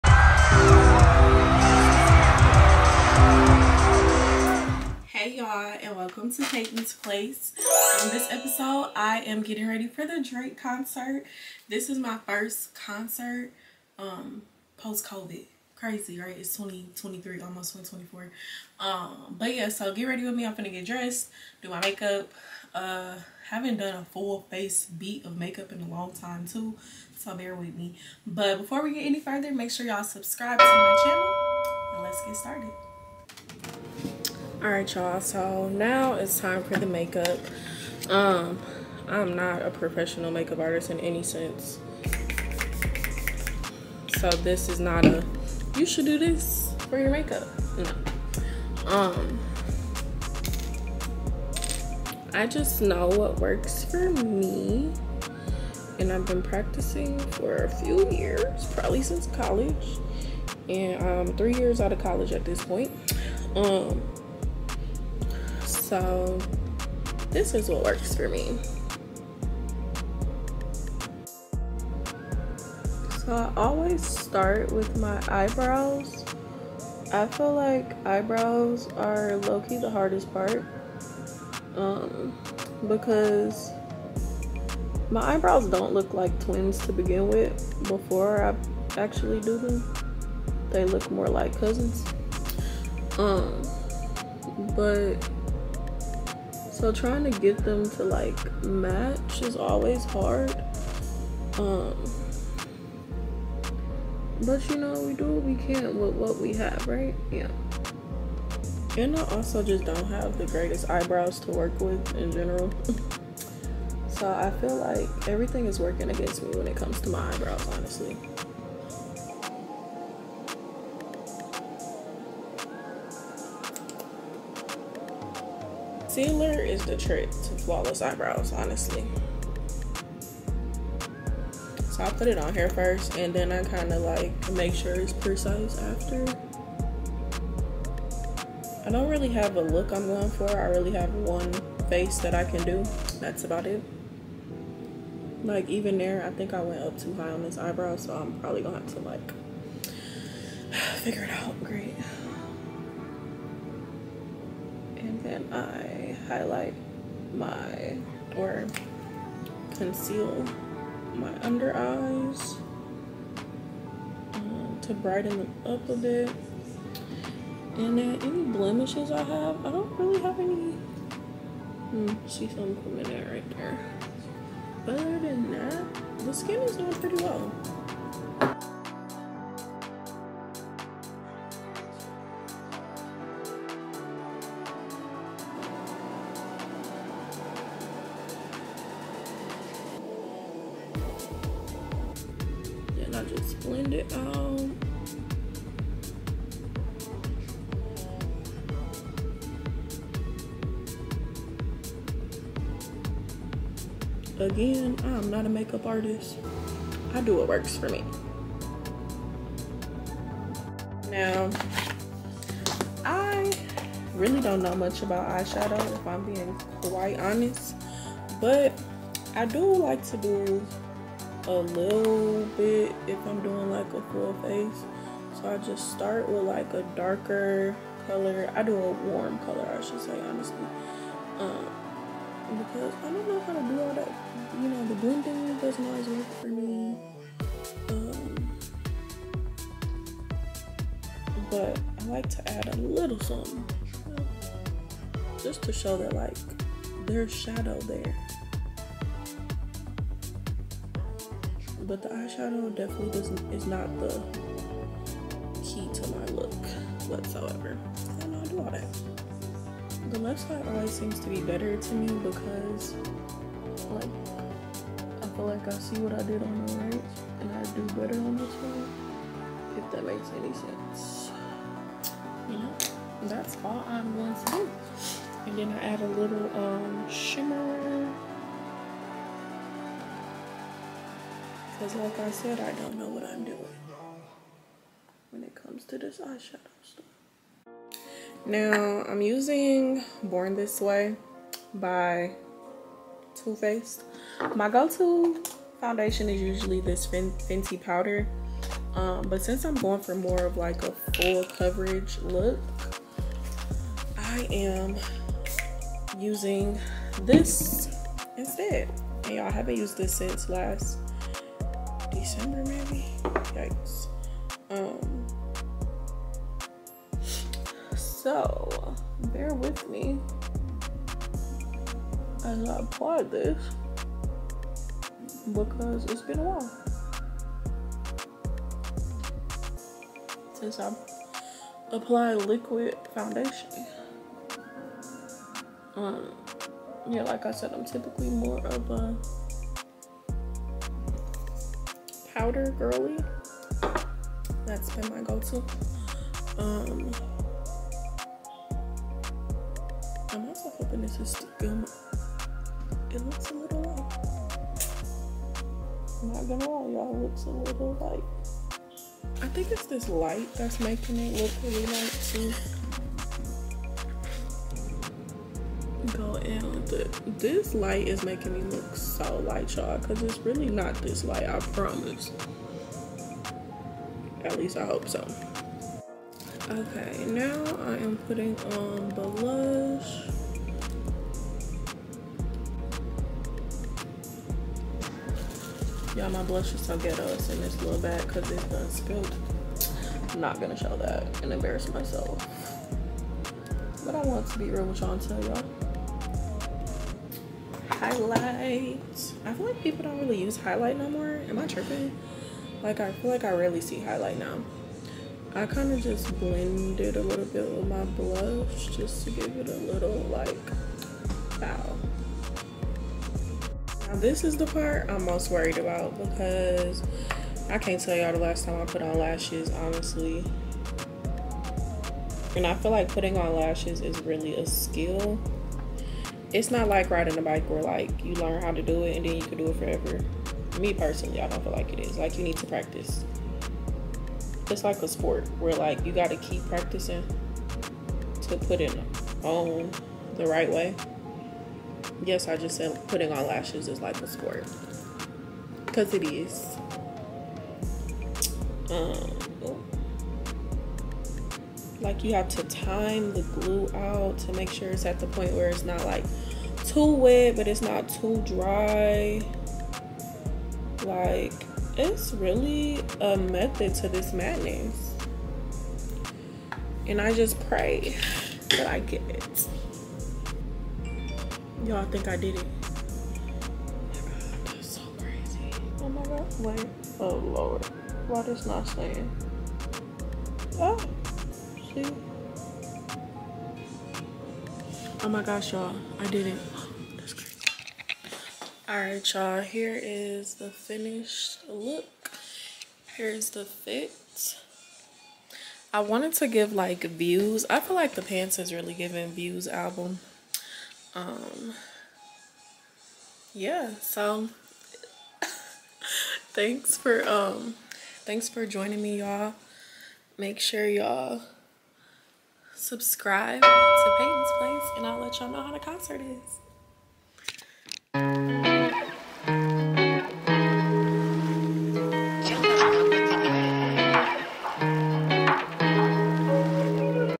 Drake Concert | Chit Chat GRWM: Secrets Revealed
In this exciting video, join me as I prepare to attend the Drake It's All a Blur Tour ! 🎤🌟 Whether you're a die-hard Drake fan or just excited for a night of great music, this GRWM (Get Ready with Me) session has got you covered.

I'll be sharing my step-by-step process for achieving the perfect concert look, from makeup to my outfit selection. Can you guess which album I dressed as?
Join me for this fun and informative GRWM session for beginners who aren't makeup experts, but still know how to make a look come together flawlessly. 🎶💄👗